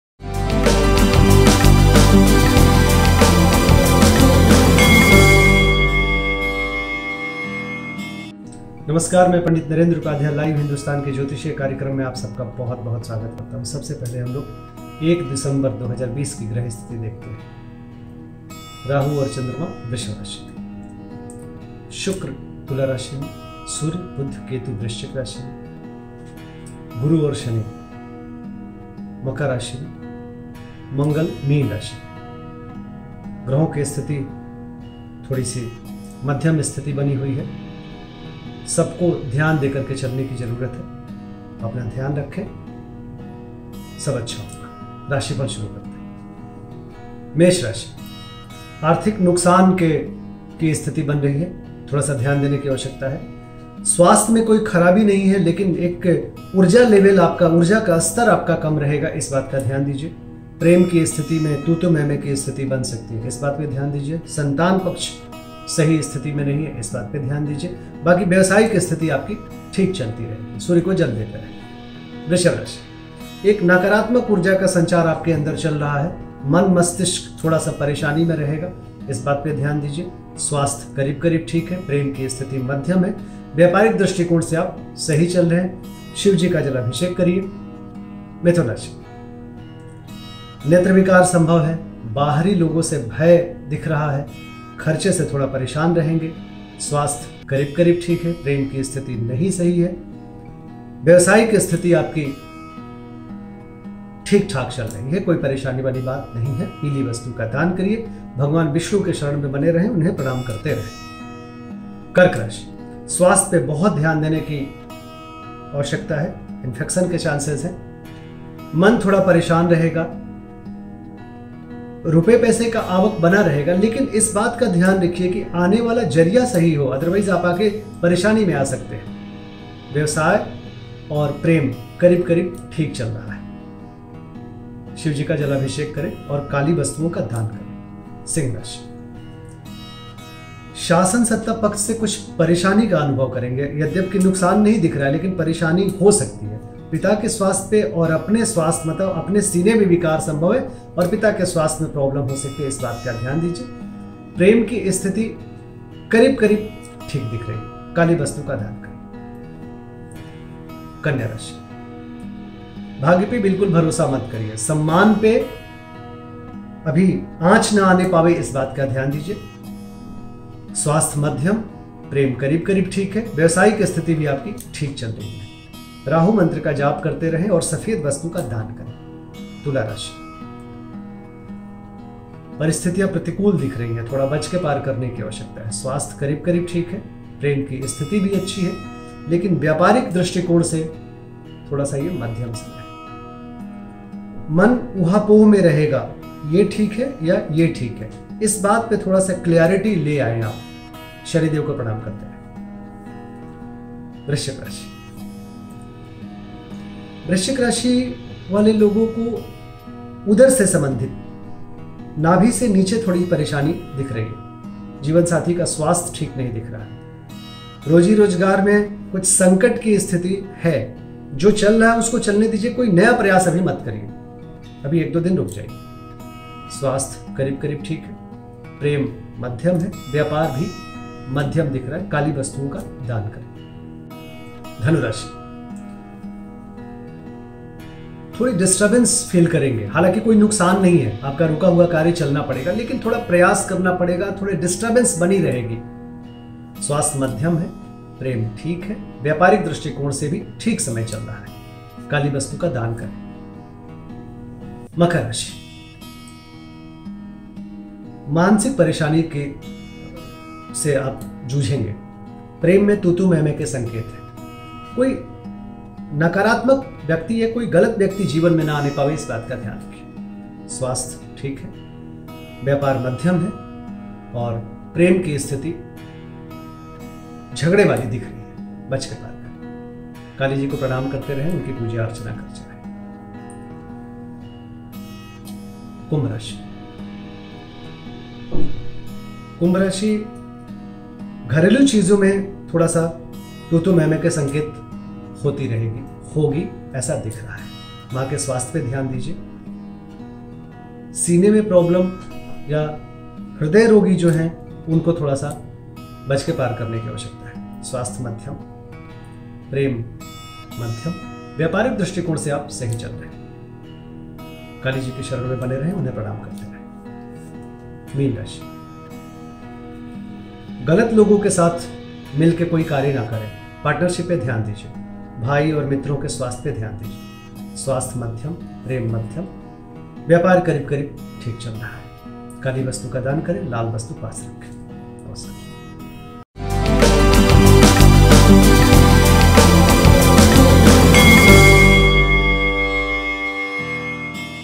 Aaj ka Rashifal, 1 December 2020, जानें कैसा रहेगा आपका 1 दिसंबर का दिन
ग्रहों की स्थिति-राहु और चंद्रमा वृषभ राशि में हैं। शुक्र तुला राशि में हैं। सूर्य, बुध, केतु वृश्चिक राशि में हैं। गुरु और शनि मकर राशि में हैं। मंगल मीन राशि में हैं। ग्रहों की थोड़ी मध्‍यम स्थिति बनी हुई है। सबको ध्‍यान देकर चलने की जरूरत है। अपना ध्‍यान रखें। सब अच्‍छा होगा।